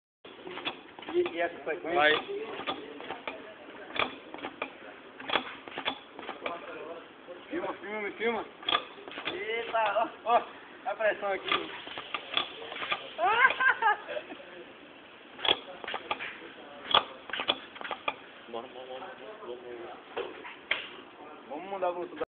trabalho de pneumática no senai
participações : Max Ian, ramón e Anderson desenvolvendo sistemas pneumaticos no laboratorio.